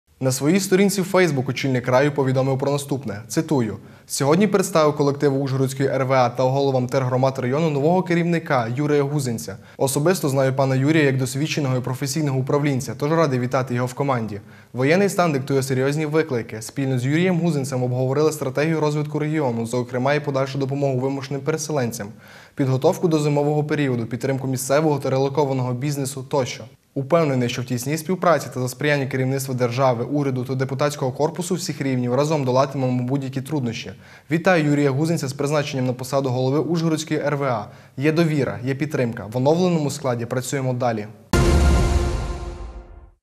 Новий керівник
На своїй сторінці в фейсбук очільник краю повідомив про наступне, цитую: Сьогодні представив колективу Ужгородської РВА та головам тергромад району нового керівника – Юрія Гузинця. Особисто знаю п. Юрія як досвідченого і професійного управлінця, тож радий вітати в команді.
Воєнний стан диктує серйозні виклики. Спільно з Юрієм Гузинцем обговорили стратегію розвитку району, зокрема й подальшу допомогу вимушеним переселенцям, підготовку до зимового періоду, підтримку місцевого та релокованого бізнесу тощо. Упевнений, що в тісній співпраці та за сприяння керівництва держави, уряду та депутатського корпусу всіх рівнів разом долатимемо будь-які труднощі.  Вітаю Юрія Гузинця з призначенням на посаду голови Ужгородської РВА. Є довіра, є підтримка. В оновленому складі працюємо далі.